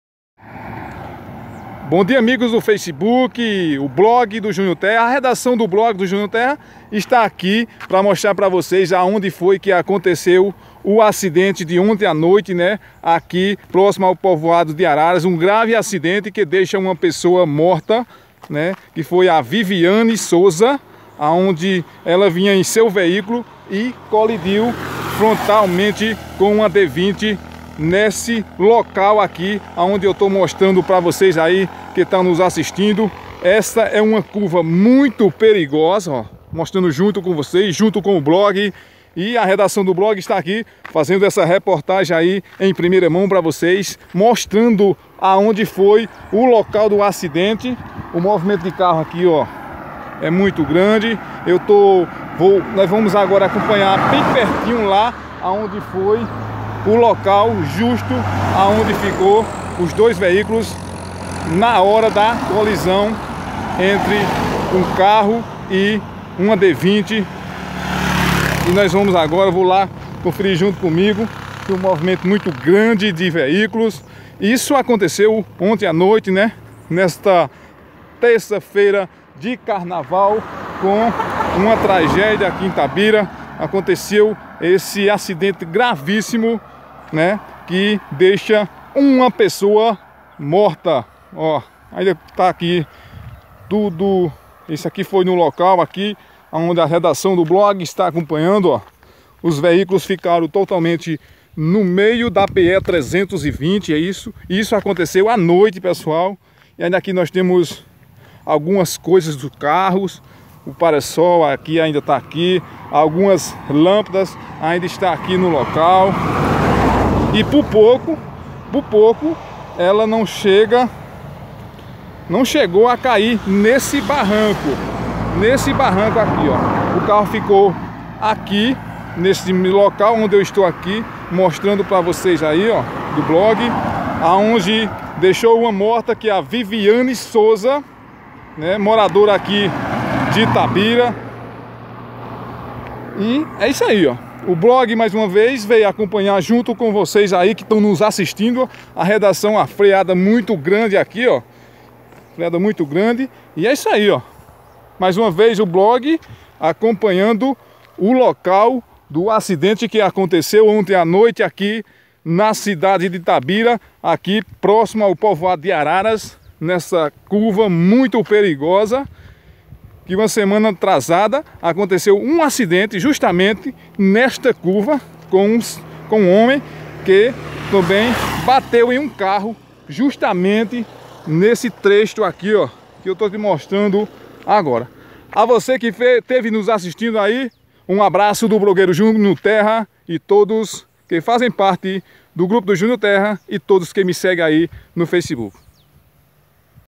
Reportagem do Blog JT Notícias no Local Do Acidente Na PE 320 Próximo a Povoado de Arara.
Vídeo de Junior Terra